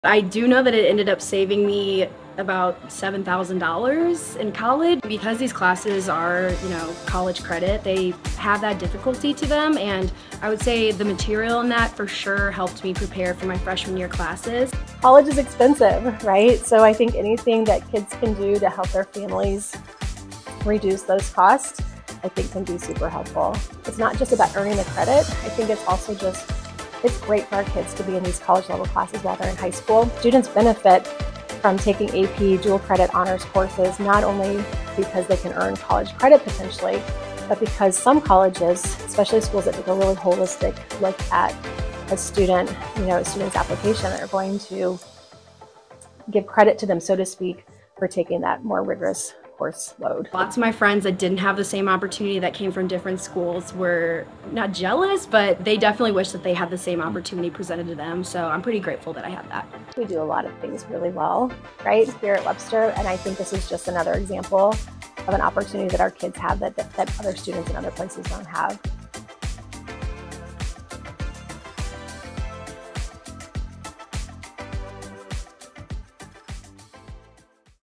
AP and Dual Credit
Learn about AP and Dual Credit courses and how they can help your WGSD student.